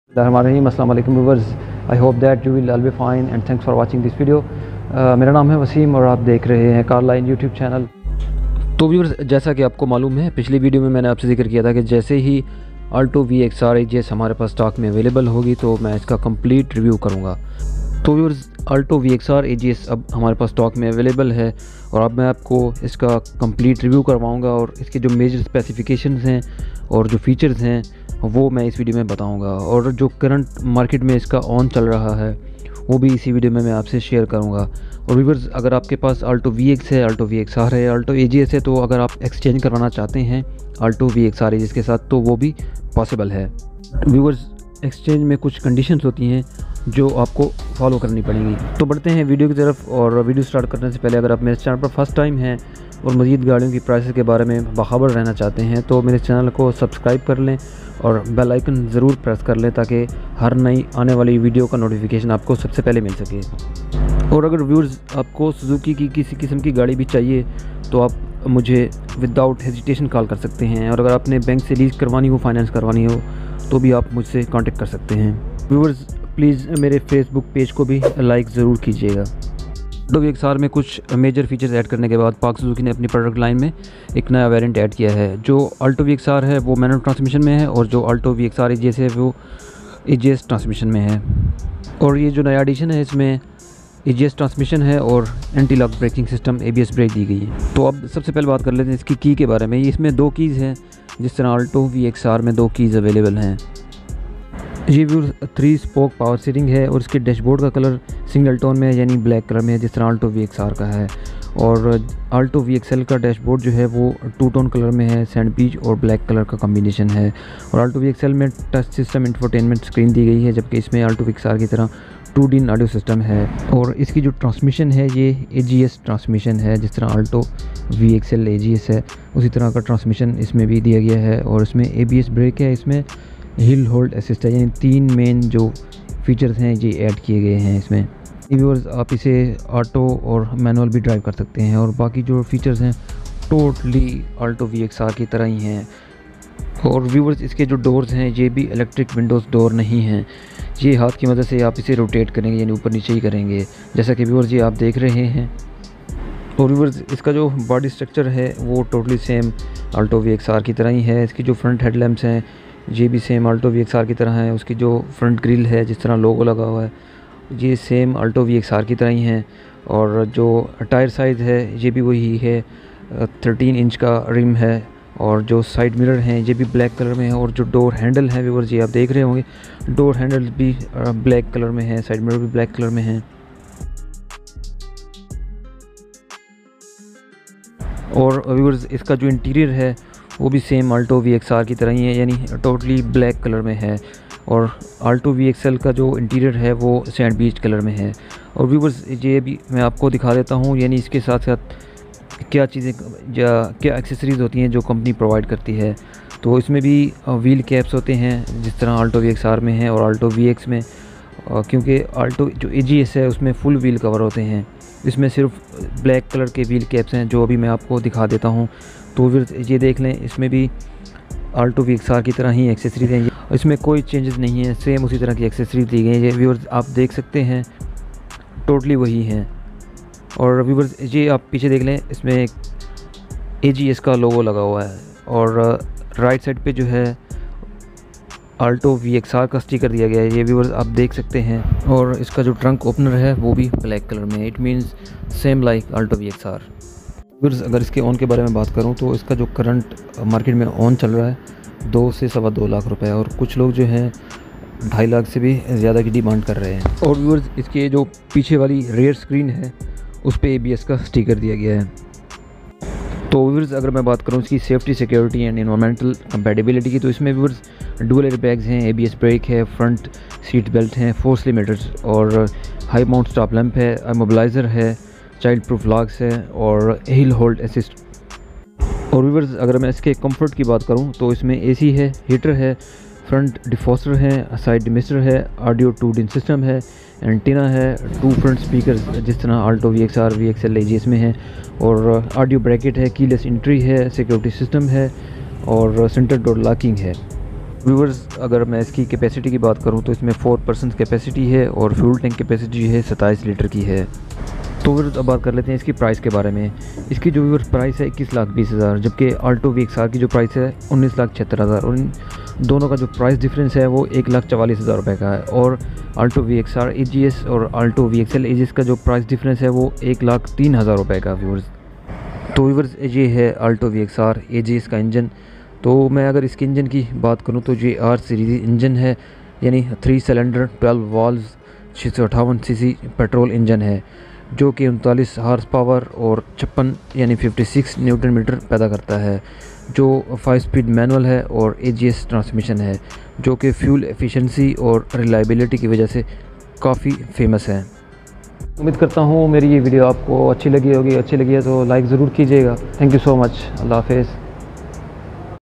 Suzuki Alto VXR Ags Complete Review Price,Specs,Features | Alto VXR Ags Auto Gear Shift | Alto 2022
Aslam o Alaikum! Dear Viewers !
The Pak Suzuki has introduced the New variant in Alto ptoduct line,
Alto vxr ags is now launched & ready to sale in market.
The customers has option to exchange their old used car like alto wagonr cultus with newly launched Alto Vxr Ags 2022 .
ALTO VXR AGS WITH ABS & Hill Hold Assist .